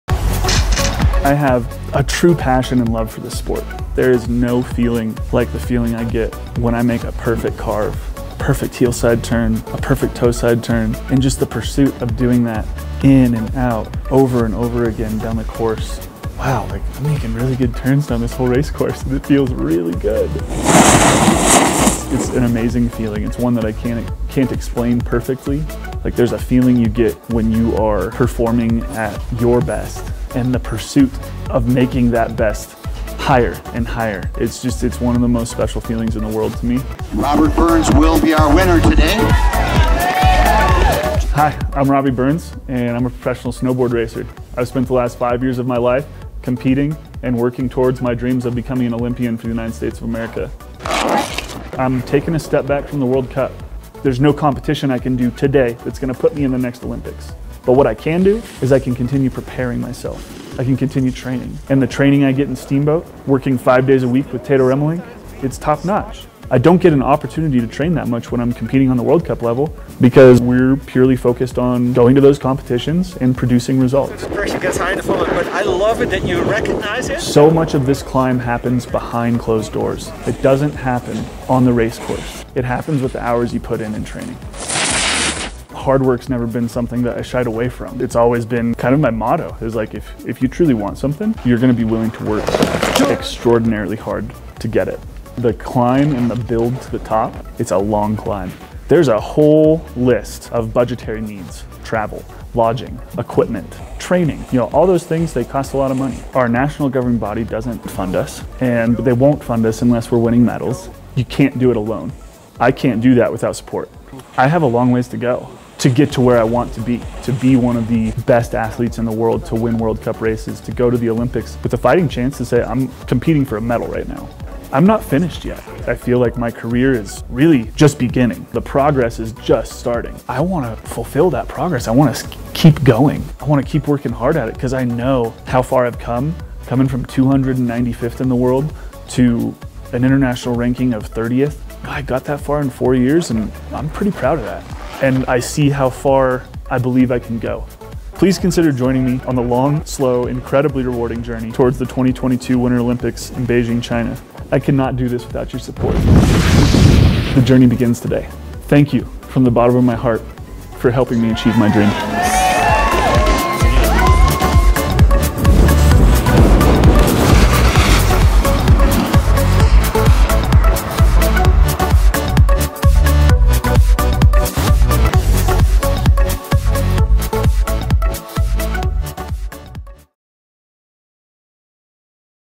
Robby Burns 2019 Olympic Dreams, Hard Work and Fundraising
Please help our dear friend and very talented athlete Robby Burns in his quest to make it to the Beijing Olympic Winter Games to race for the USA, there is no one nicer and more giving than Robby and he always makes us proud. Robby is taking a job with another team this season but he remains one of our teammates always and will still stay connected with the team, we always wish him the best of success no matter where he goes and he will always have a home in Steamboat!